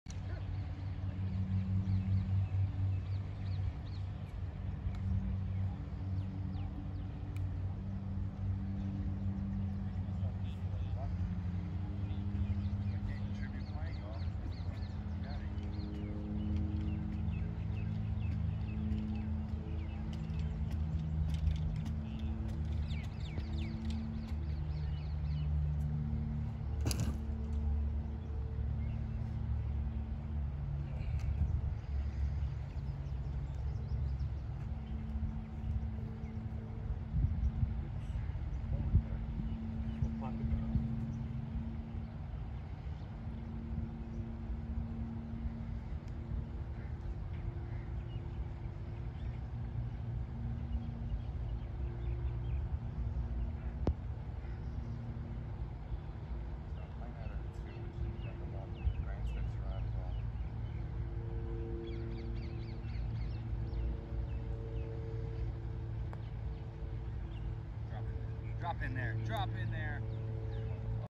Erik van Rooyen Putting Master Class
Think you've got an elaborate pre-shot routine? Check out EvR at the Dubai Desert Classic, January 2020.